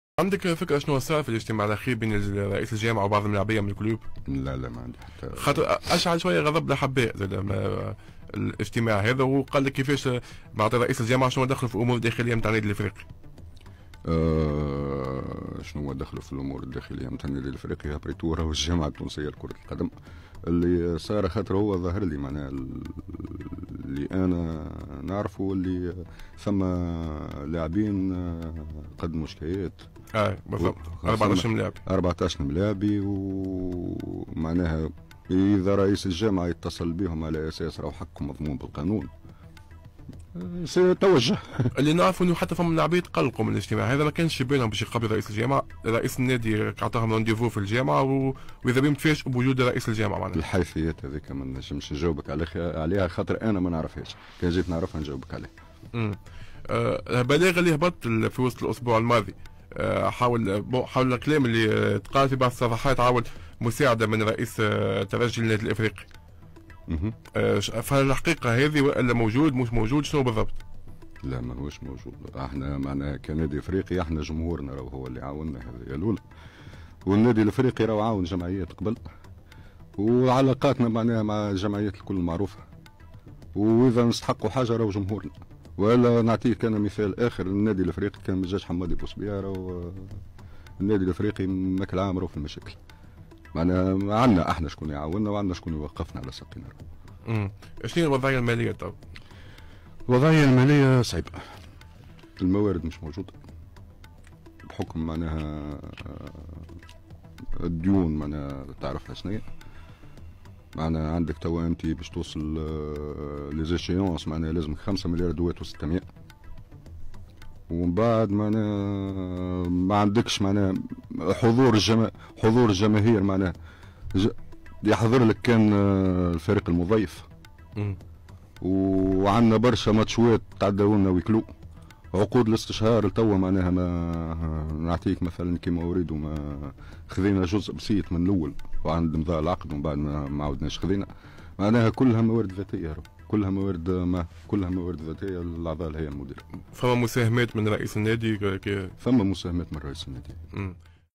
سامي المقدمي يوضح حقيقة المبلغ الذي تبرع به حمدي المؤدب للافريقي وأسباب زيارة اللاعبين لرئيس الجامعة
سامي المقدمي يوضح حقيقة المبلغ المالي الذي تبرع به حمدي المؤدب للافريقي وأسباب زيارة اللاعبين لرئيس الجامعة